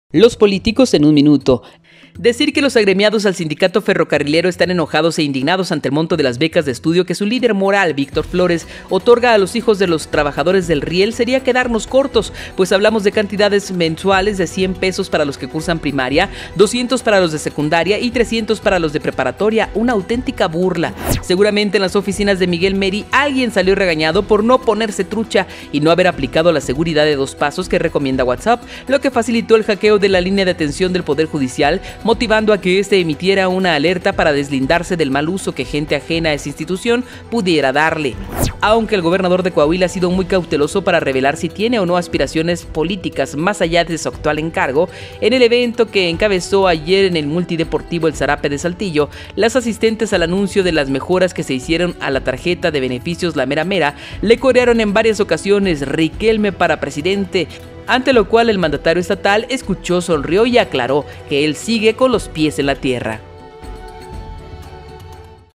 LOS POLÍTICOS - Es una burla 🖊️
Al interior del gremio de trabajadores ferrocarrileros hay una gran inconformidad hacia su líder moral, Víctor Flores; en otro tema el Poder Judicial de Coahuila emitió ayer una alerta a la población para advertir que el número de WhatsApp por el que tenían comunicación directa con miles de usuarios, fue comprometido... de esto y mucho más en la columna LOS POLÍTICOS de EL DIARIO de Coahuila